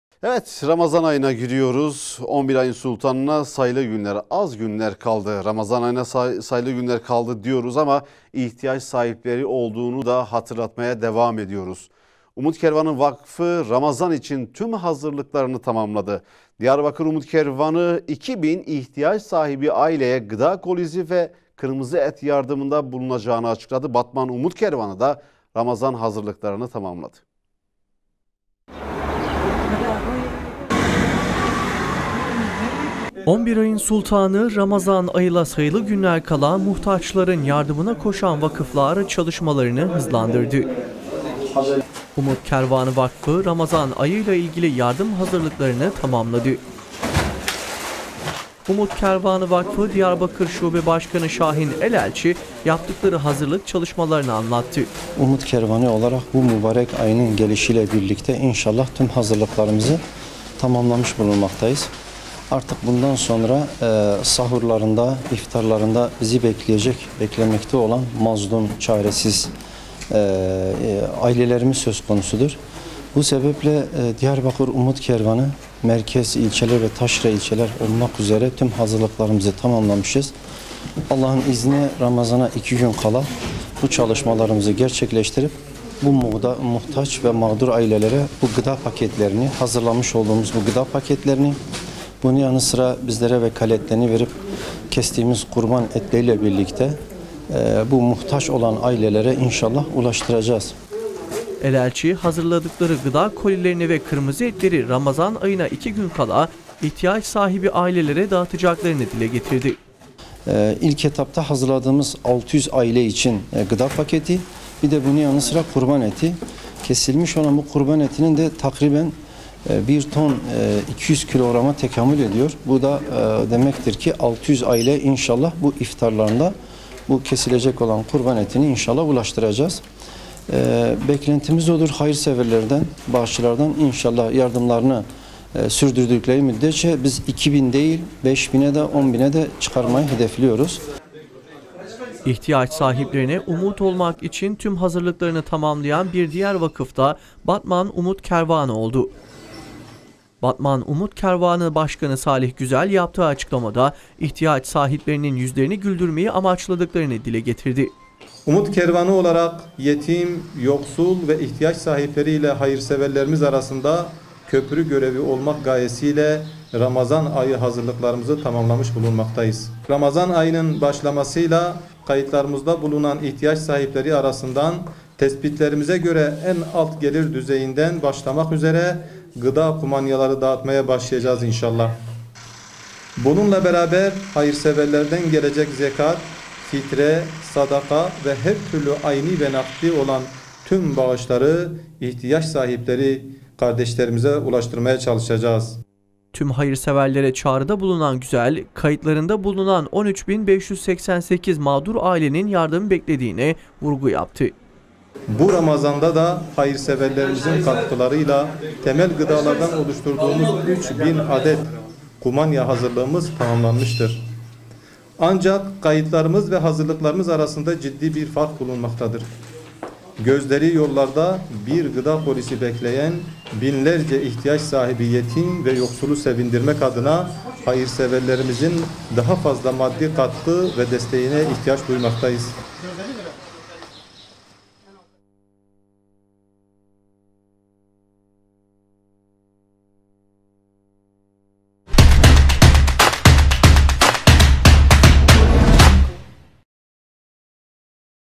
Umut Kervanı Ramazan’a Hazır
Ramazan ayına sayılı günler kaldı. Umut Kervan Vakfı Ramazan için tüm hazırlıklarını tamamladı. Diyarbakır Umut Kervanı, 2 bin ihtiyaç sahibi aileye gıda kolisi ve kırmızı et yardımında bulunacağını açıkladı… Batman Umut Kervanı da Ramazan hazırlıklarını tamamladı.

   OYNATMA LİSTELERİMİZ